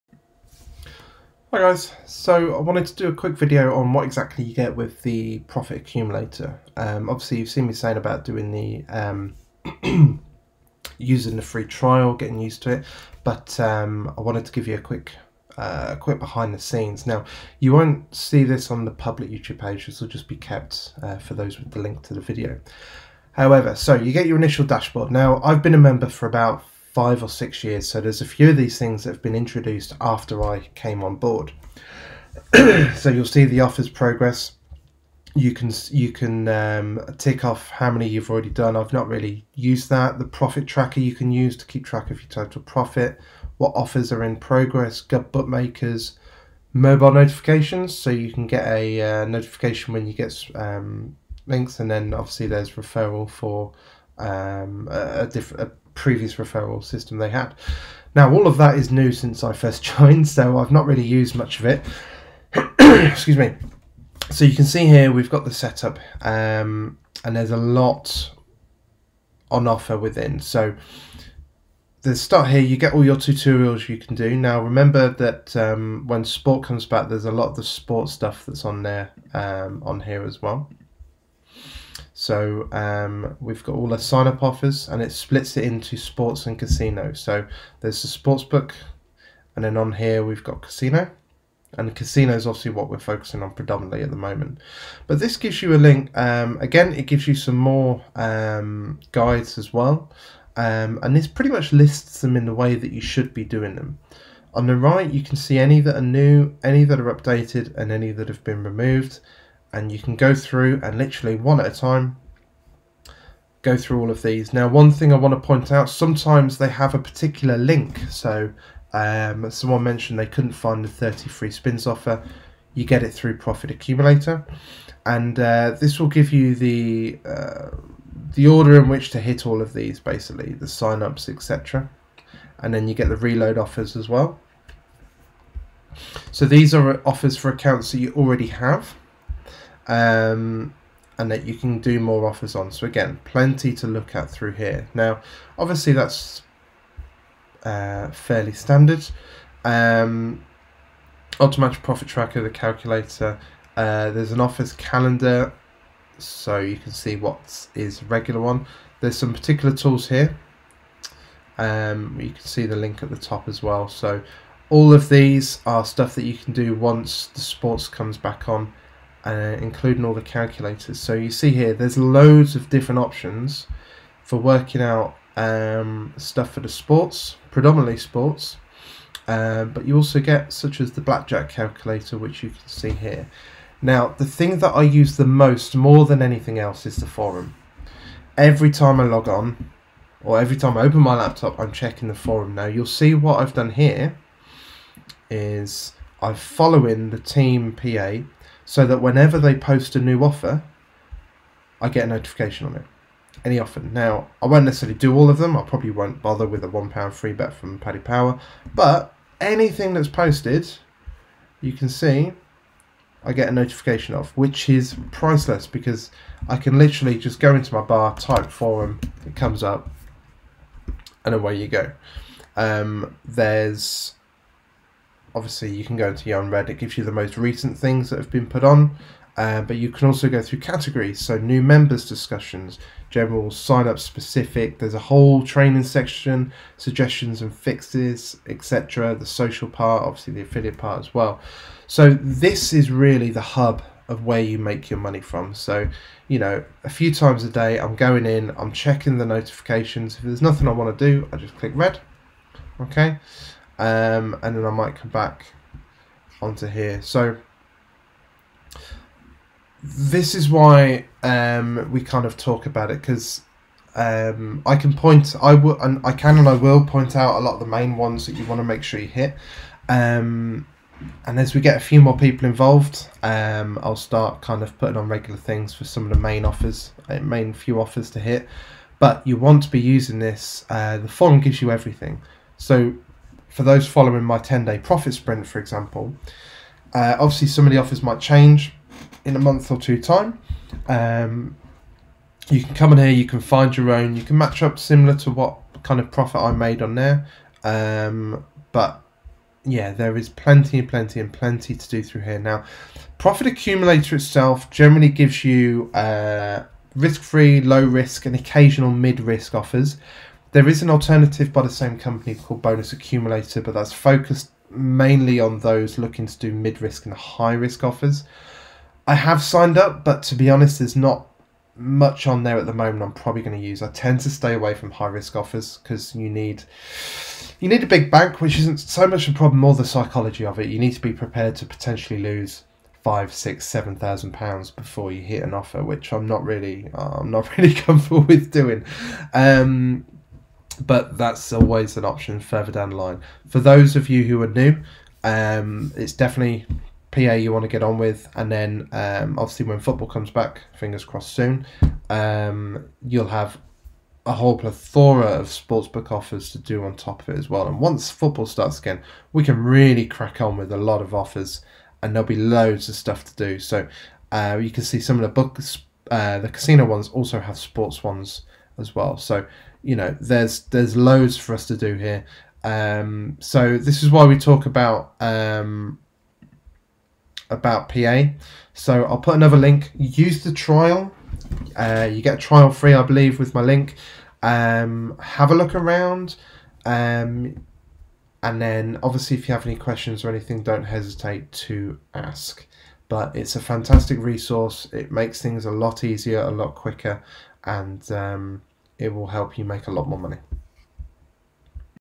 What is Profit Accumulator? How does it help you make profit?
PA, and in particular the forum, is a huge part of continuously making profit with casino and sports offers.